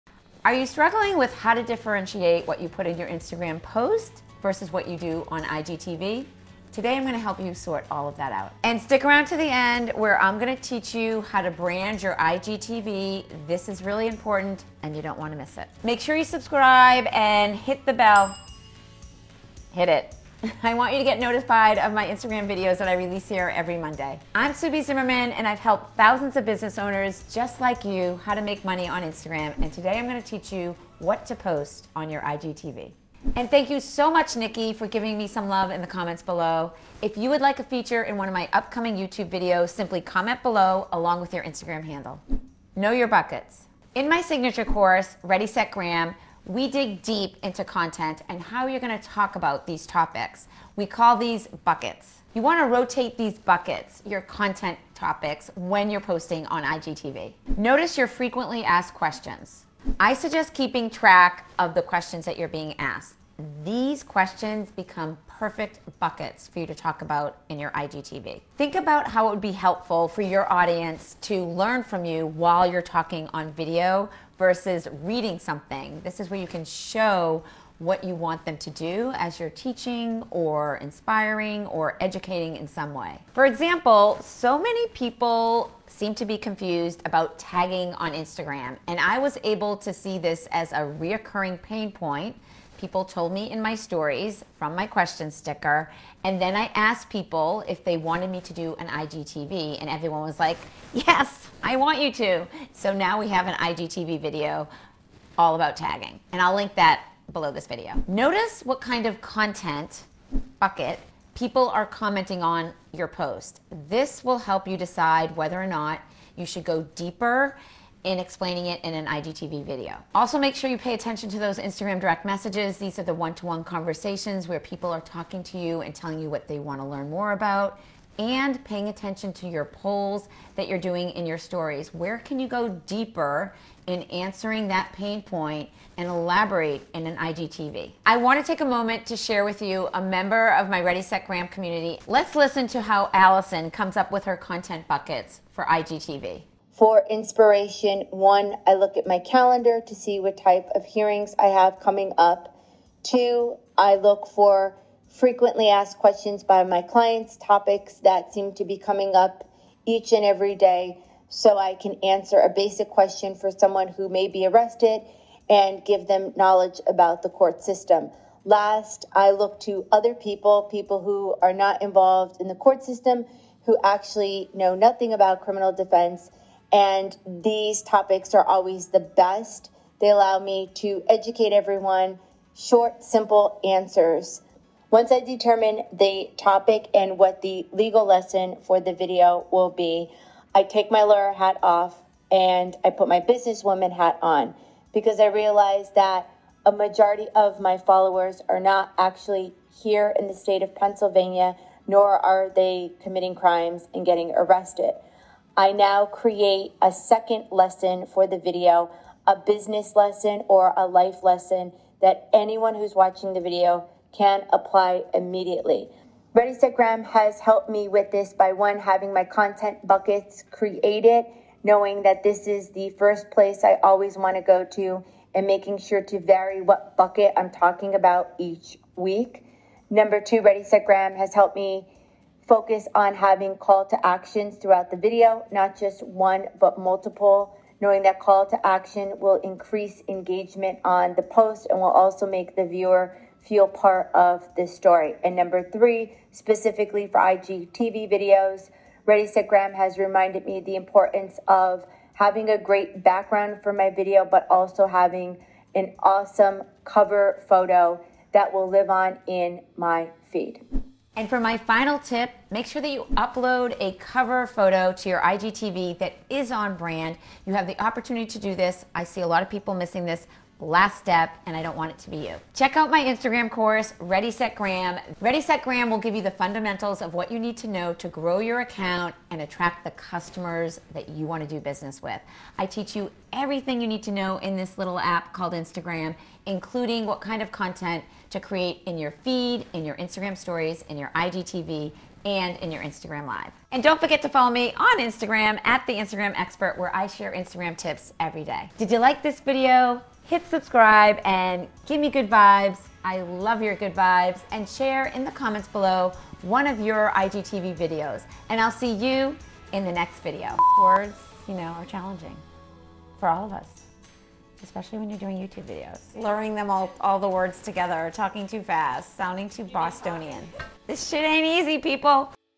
What To Post On IGTV (HOW TO FIGURE OUT WHAT TO TALK ABOUT)
FREE IG TRAINING: The 3 keys to getting engaged followers and buyers

Prefer to read? Here is the blog post

Sue B the Instagram Expert shares how to know what to post on IGTV. It's hard to know what to talk about on video on instagram. If you are wanting to use IGTV or Instagram tv, sue b instagram will teach you the igtv tips so you create igtv videos.